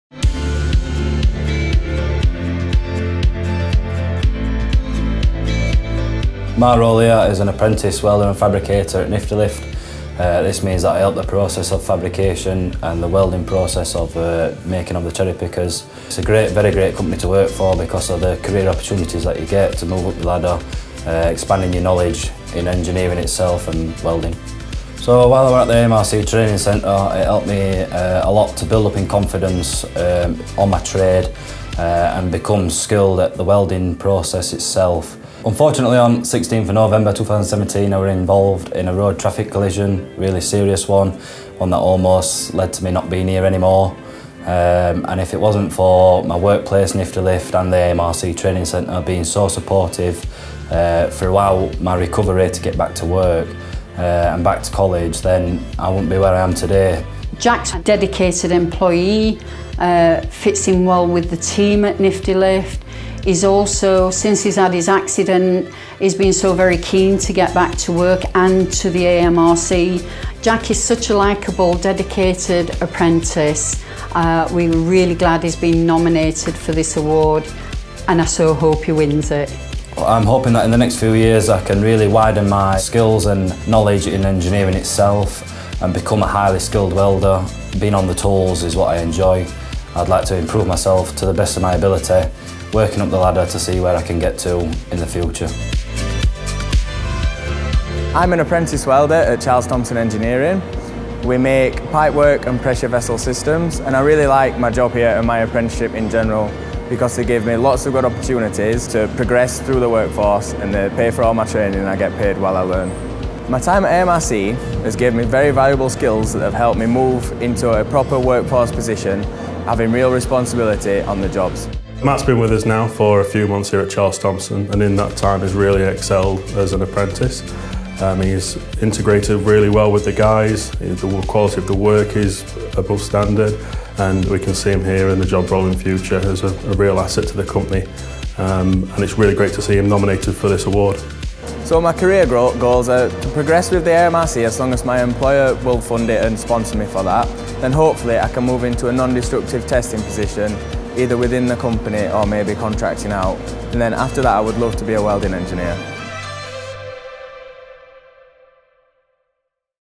2019 Nominees of the Fabrication and Welding Apprentice of the Year Award
Meet Jack and Matthew, two of our Fabrication and Welding apprentices at the AMRC Training Centre, and find out about their current roles and career aspirations.

Find out more about how you can get started in Fabrication and Welding